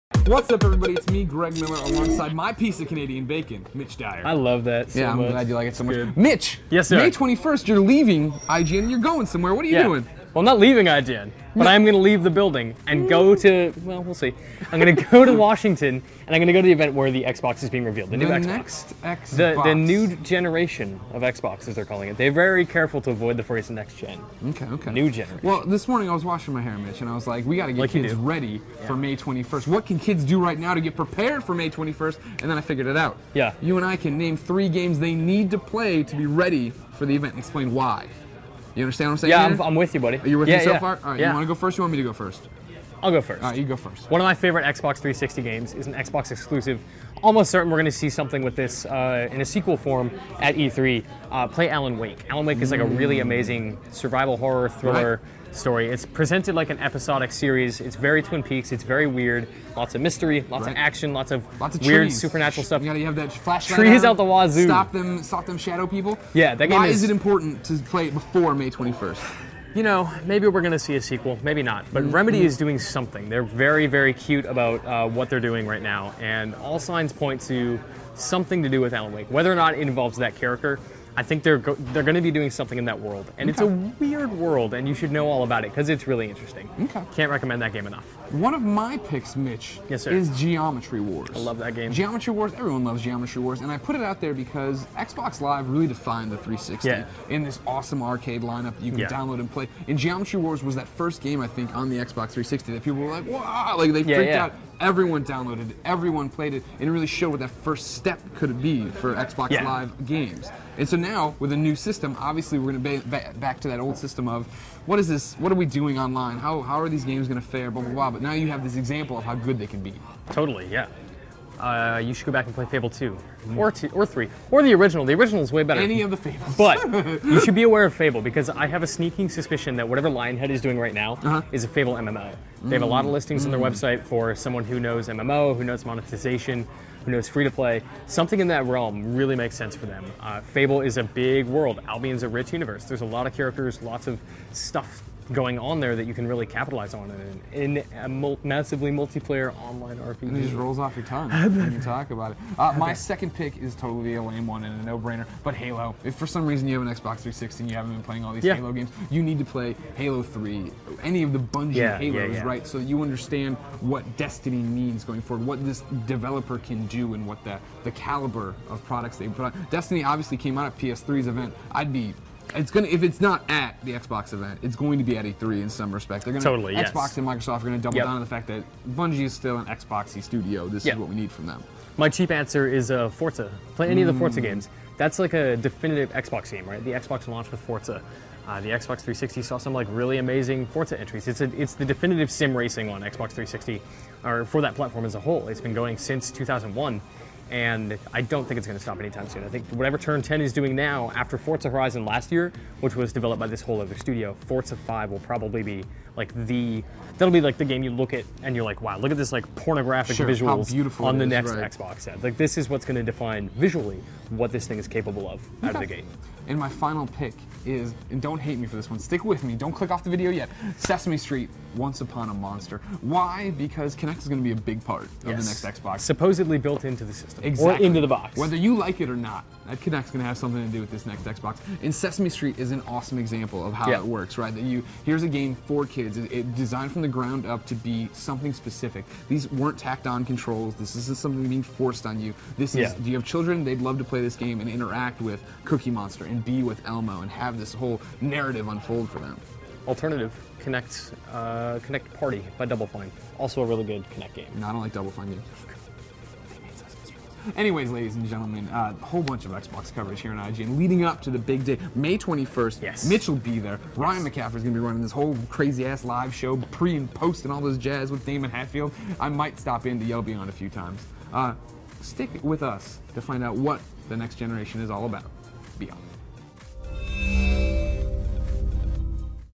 The Games You Need to Play Before the Xbox Event - IGN Conversation
Before the next Xbox hits on May 21st, you have to play these games to truly be ready.

The Games You Need to Play Before the Xbox Event